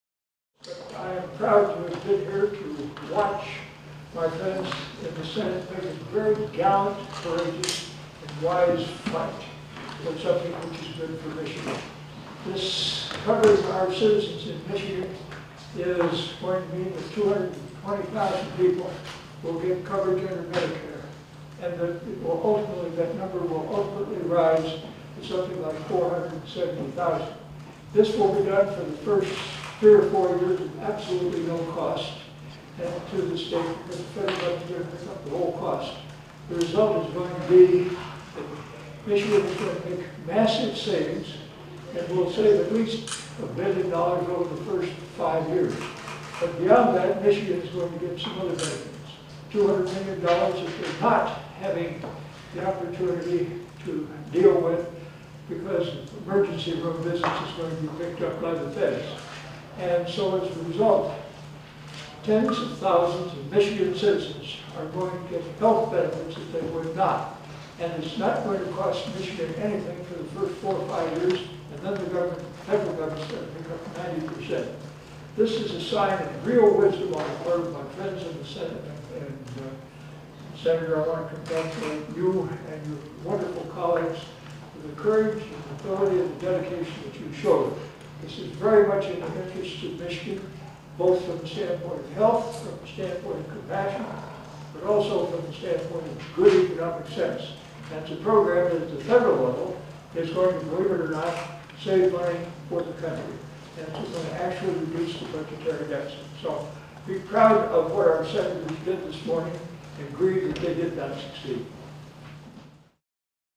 Congressman John Dingell (D-Mich.) Speaks in Support of Michigan Senate Democrats
The Michigan Senate Democrats have been calling for an expansion of Medicaid as part of the Affordable Care Act since 2012.

Expanding Medicaid in Michigan would extend health care coverage to Michigan residents who are under 133% of the federal poverty level. In total, 320,000 Michiganders would be covered in the first year of the expansion, and an estimated 470,000 would be covered by 2021. Michigan's uninsured population will drop by approximately 46% if the expansion is enacted.

House Bill 4714 to expand Medicaid in Michigan was introduced in May, and was passed with bipartisan support in the Michigan House of Representatives on June 13, 2013. Governor Snyder, along with medical and business professionals, has championed the legislation. Unfortunately, Senate Republicans have opted to take a two month summer break rather than bringing the legislation up for a vote. Senate Democrats will be in session and ready to vote on the expansion on July 3rd, and are encouraging the people of Michigan to join us in showing support for the legislation.

Medicaid expansion also includes a 48-month cap on benefits offered through this legislation. Because this differs from what is allowed under federal law, the state must seek a waiver from the federal government before the expansion can be implemented in January of 2014. In order to ensure this waiver is received in time, Michigan must submit its application as soon as possible. The longer it takes this bill to pass the Senate, the less likely it becomes that a waiver will be granted in time.

Failure to pass this bill will result in Michigan losing out on an additional $2 billion in federal Medicaid funding. It also means the state will continue to spend more than $200 million annually on emergency room visits and other health care costs for those that we failed to cover.